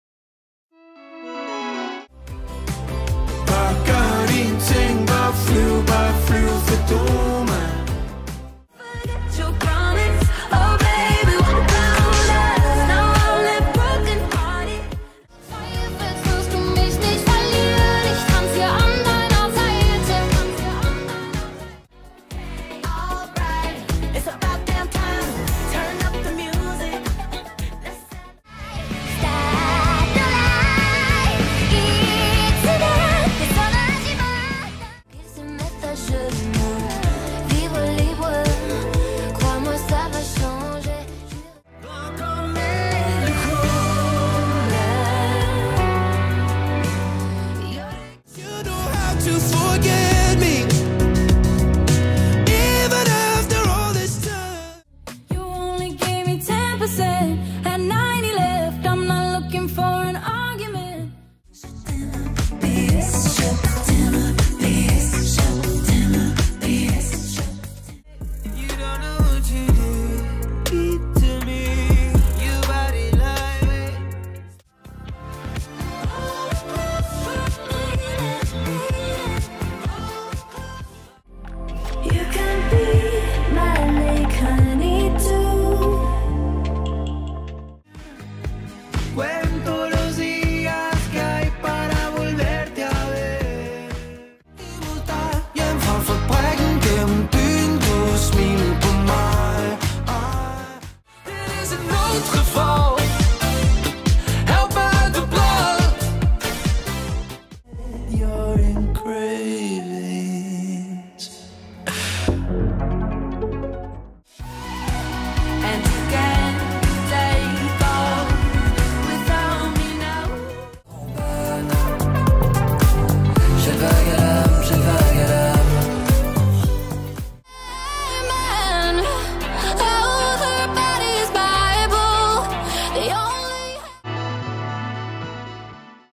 My Music Top 20 - October 2022
Hello there! This is my top 20 with the most played songs during October.